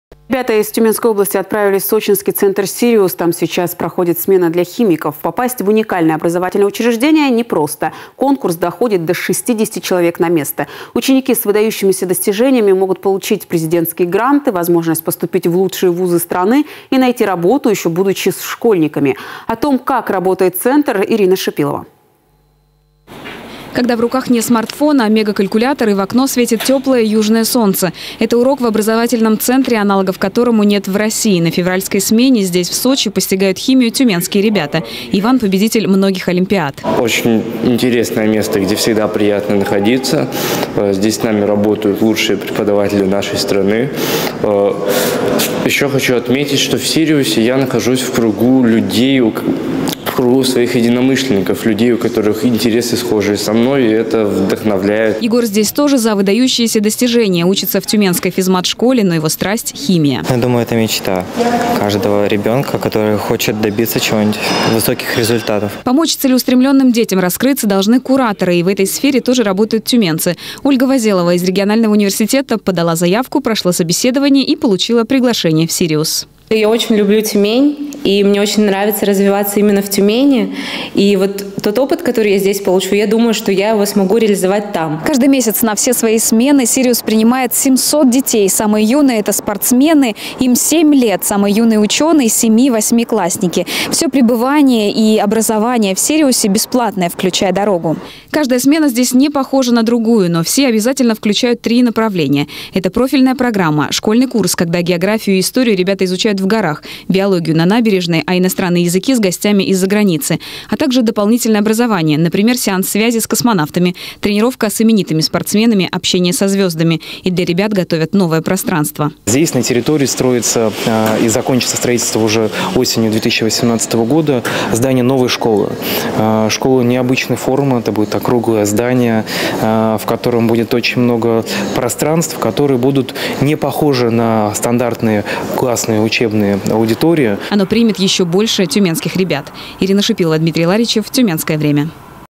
Таланты в Сочи
Ребята из Тюменской области отправились в сочинский центр «Сириус». Там сейчас проходит смена для химиков. Попасть в уникальное образовательное учреждение непросто - конкурс доходит до 60 человек на место. Ученики с выдающимися достижениями могут получить президентские гранты, возможность поступить в лучшие вузы страны и найти работу, ещё будучи школьниками.